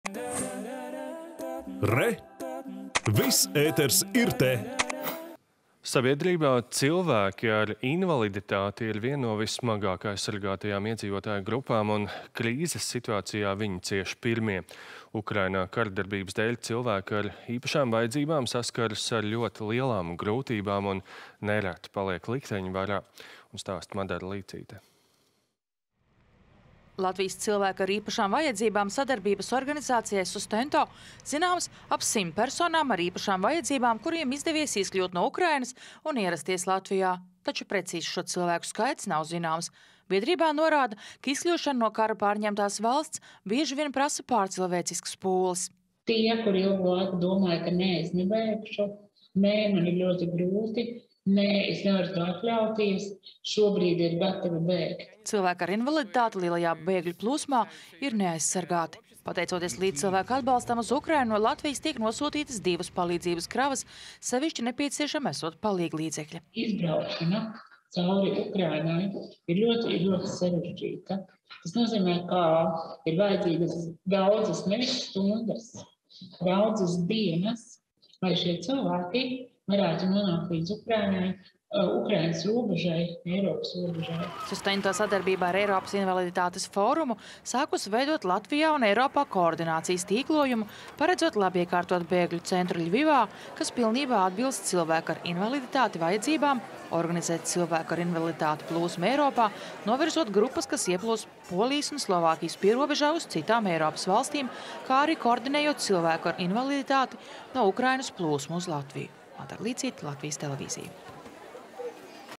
Aicina palīdzēt bēgļiem ar īpašām vajadzībām
Sabiedrībā cilvēki ar invaliditāti ir viena no vismazāk aizsargātajām iedzīvotāju grupām, un  krīzes situācijā viņi cieš pirmie. Ukrainā karadarbības dēļ cilvēki ar īpašām vajadzībām saskaras ar ļoti lielām grūtībām un nereti paliek likteņa varā. Turpina Madara Līcīte.